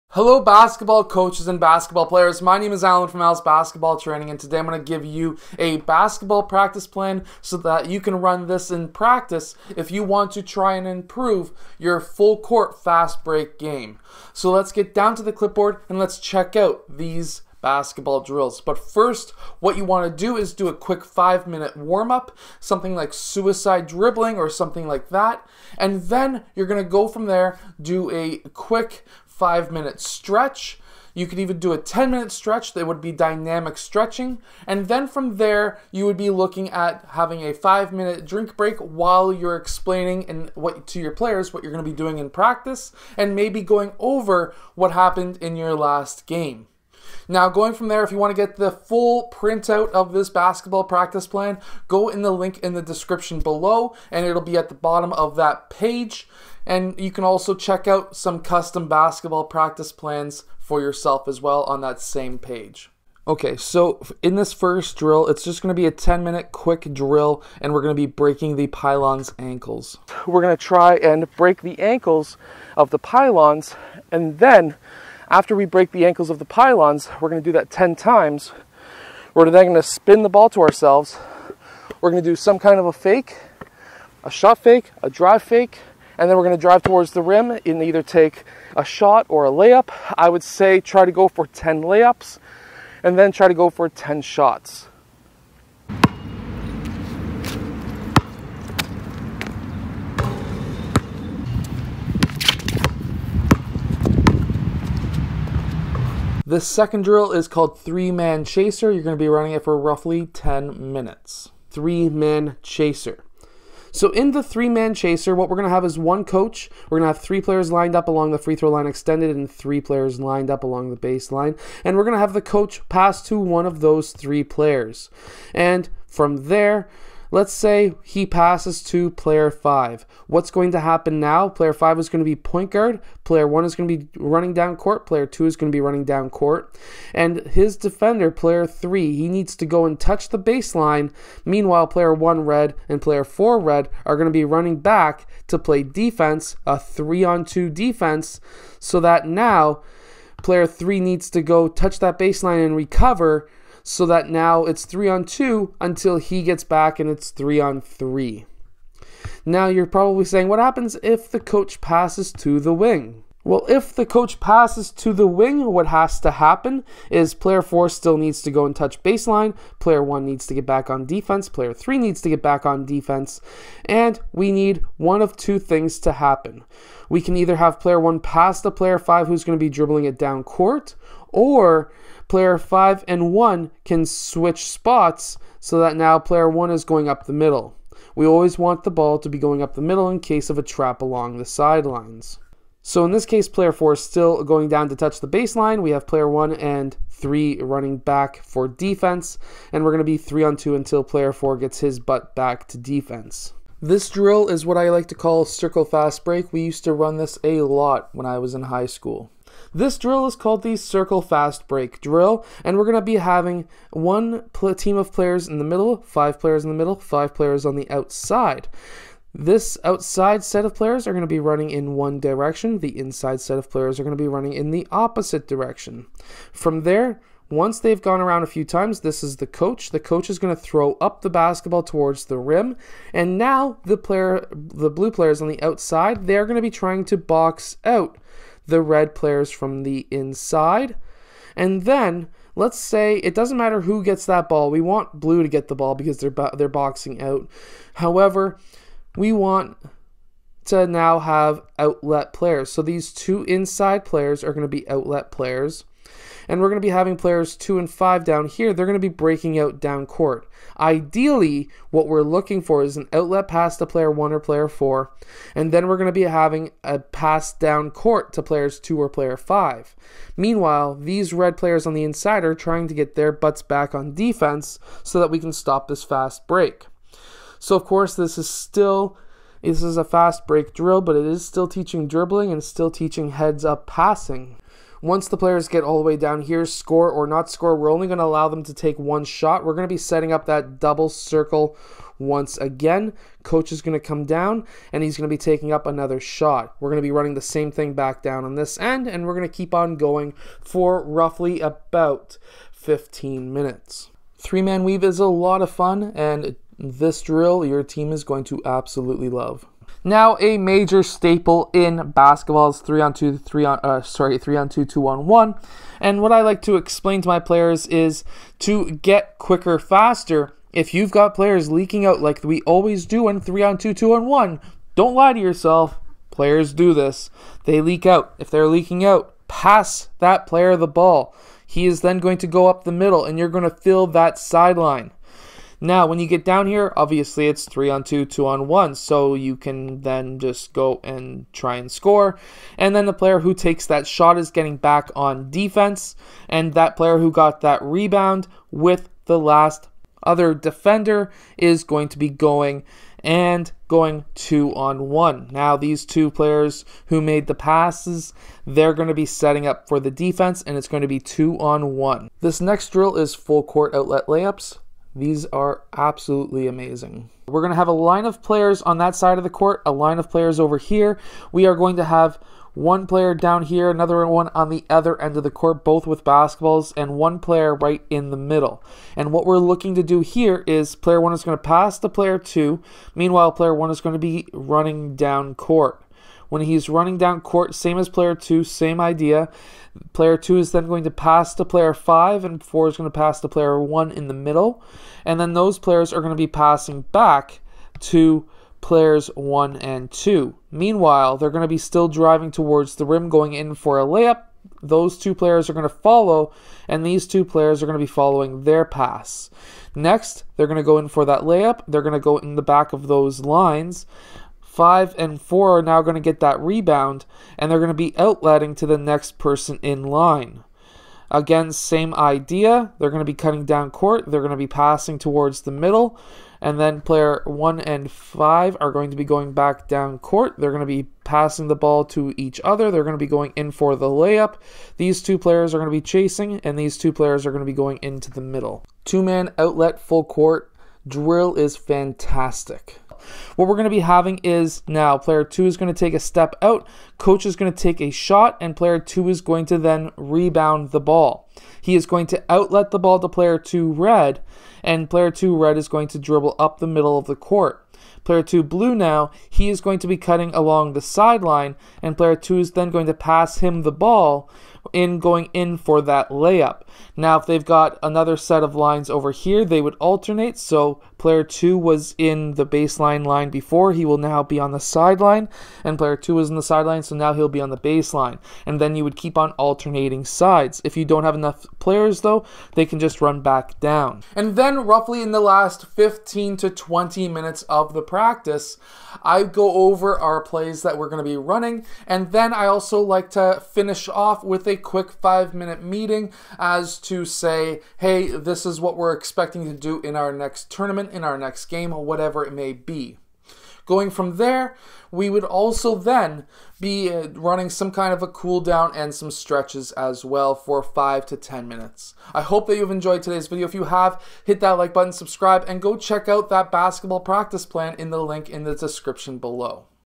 High School Basketball Practice Plan For Fast Break Basketball Drills
High School Basketball Practice Plan For Fast Break Basketball Drills, in this video I breakdown different Fast Break Basketball drills that you can run at the high School level to teach your team to push the Basketball down court. I like running a fast tempo style of game, I find this beneficial in scoring quick and easy to run the score of quickly.

Amazon links
The Basketball I use

1 on 1 Basketball Training (Hamilton Ontario and Surrounding Areas)